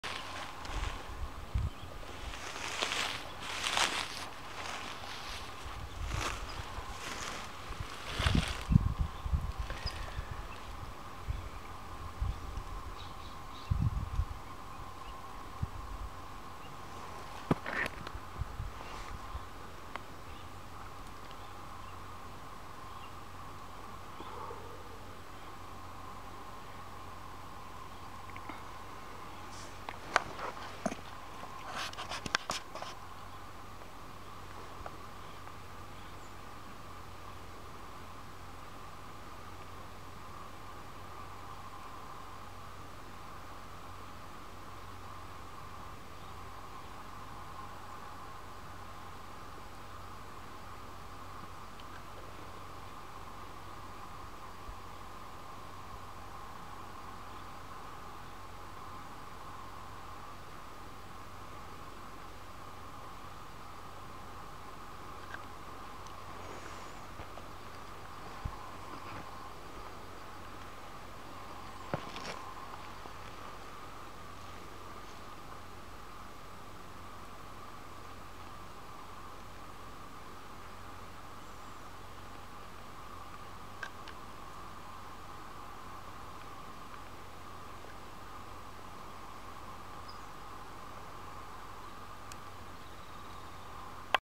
Black Tarantula Again (part 2)
More of that big black tarantula on the move. He really gets around.